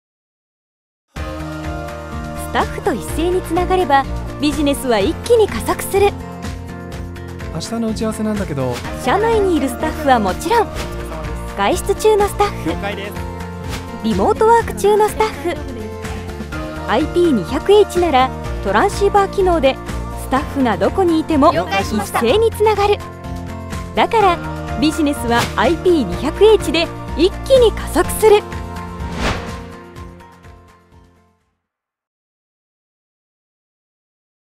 一斉につながるIP200H
無線モバイルIPフォンIP200Hなら、社内のスタッフ、出張中のスタッフ、リモートワーク中のスタッフにも、一斉に連絡することができます。

無線モバイルIPフォン IP200H 製品ページ